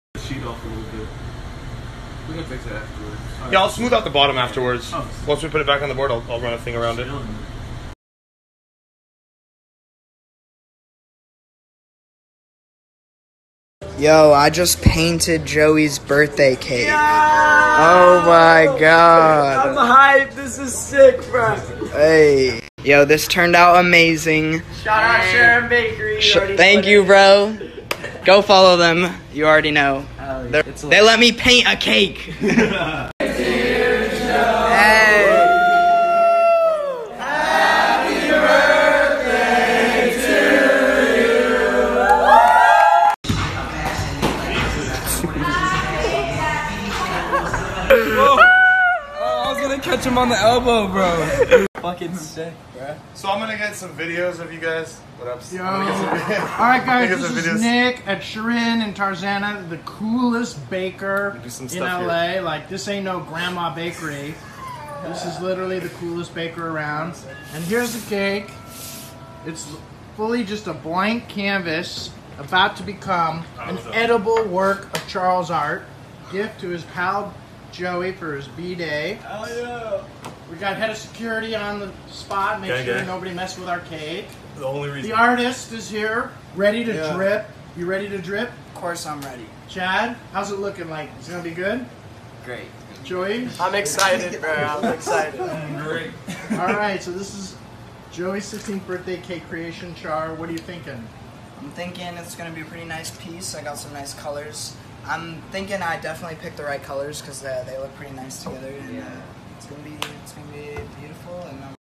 Joey Birlem's 16th Birthday Party
Joey Birlem has turned out 16. Here are some of the clips from his birthday party.